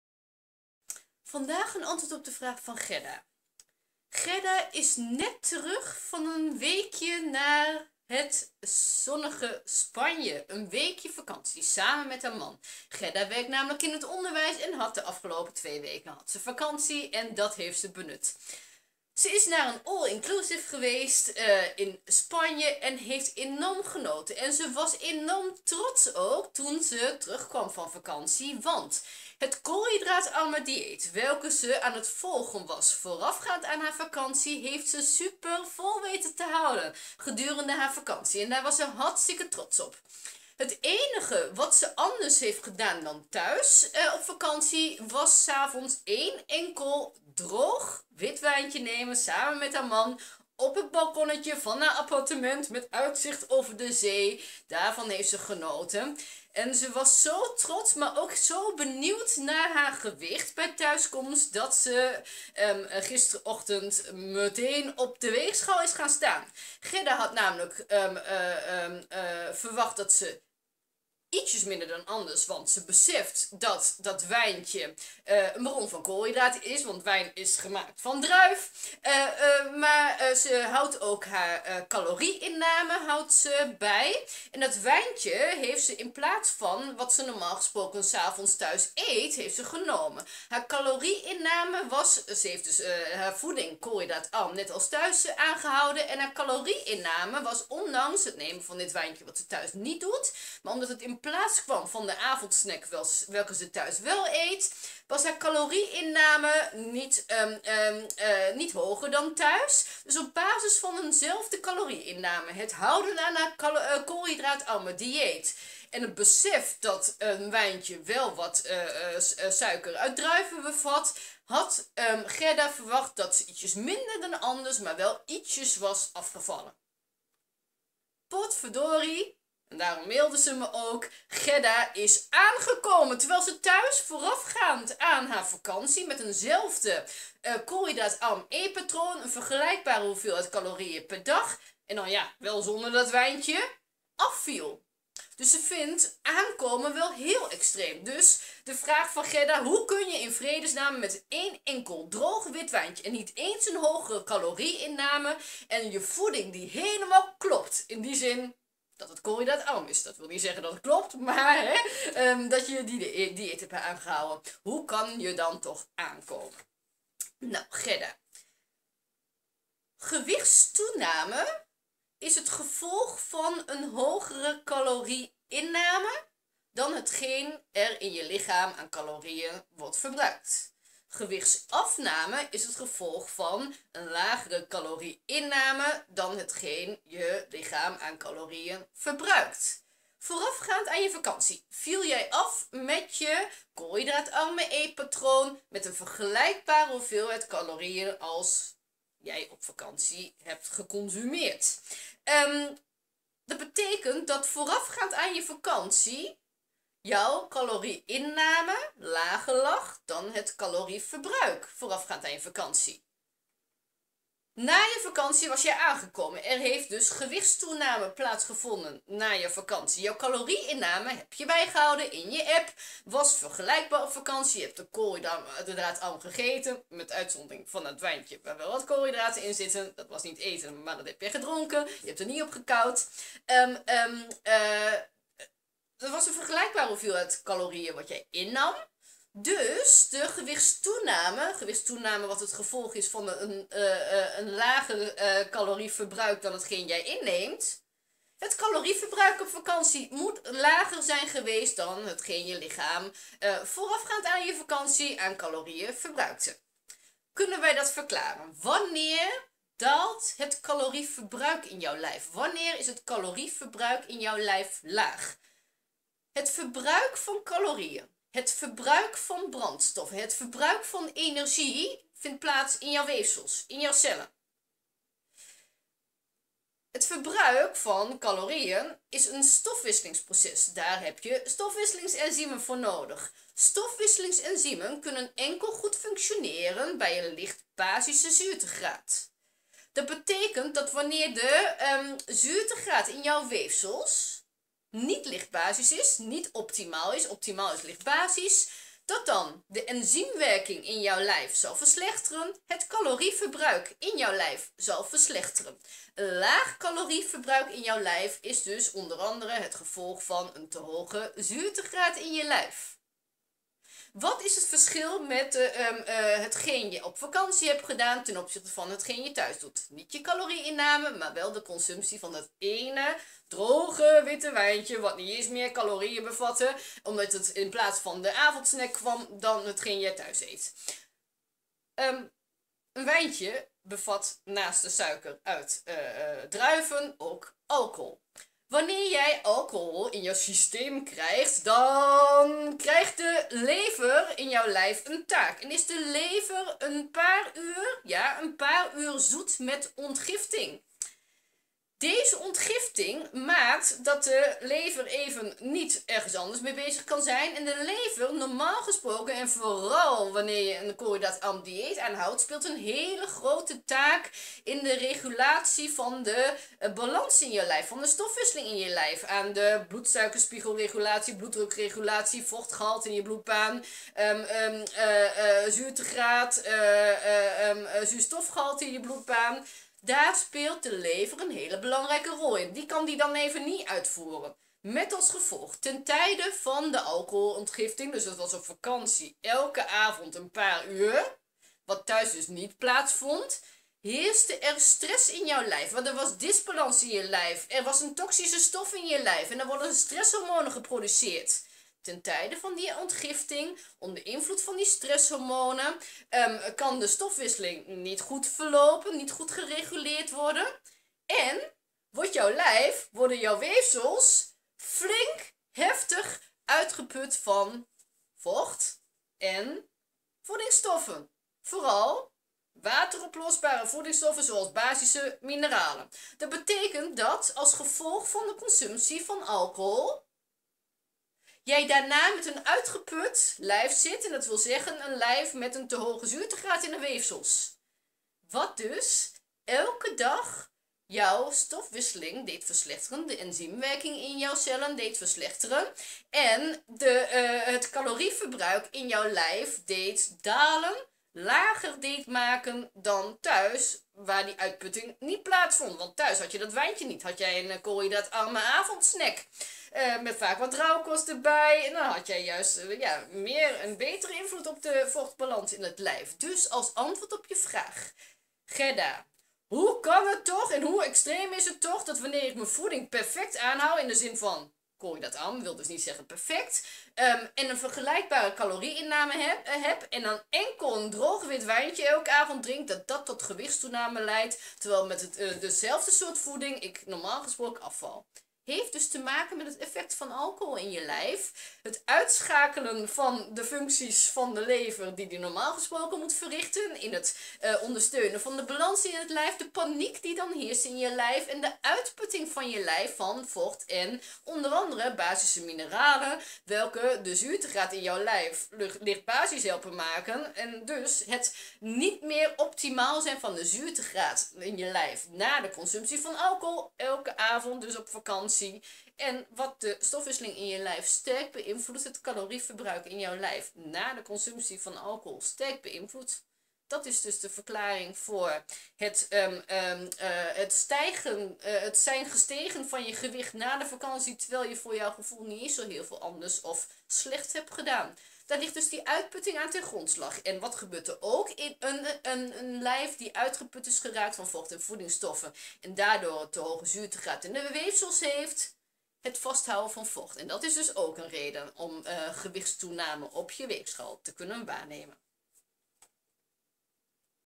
Vraag en antwoord: Je hebt je dieet aan weten te houden op vakantie en toch ben je aangekomen!
In dit filmpje leer je dat de consumptie van alcohol van invloed is op de stofwisseling in je lijf.
Je leert dat de consumptie van alcohol je lijf doet uitputten van vocht en voedingsstoffen en dat daardoor na de consumptie van alcohol het calorieverbruik in je lijf lager is dan wanneer je geen alcohol zou hebben gedronken.